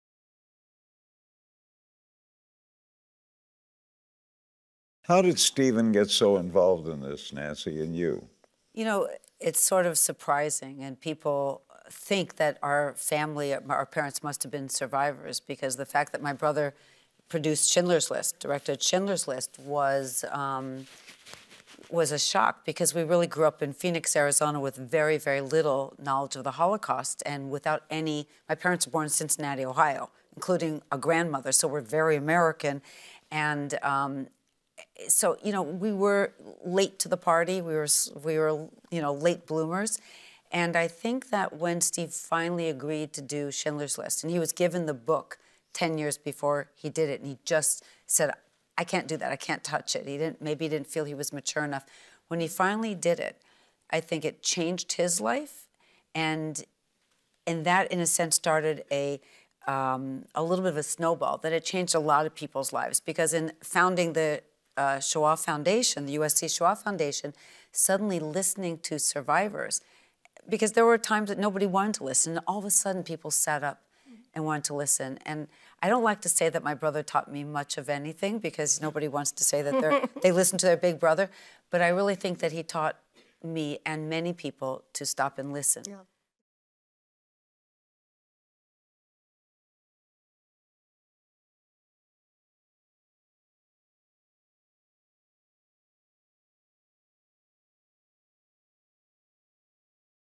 Documenting the Holocaust on Film - Nancy Spielberg on Schindler's List
At PaleyIMPACT LA on February 7, 2019, documenting the Holocaust on film, Nancy Spielberg talks about her brother Steven and the impact of his iconic film Schindler's List.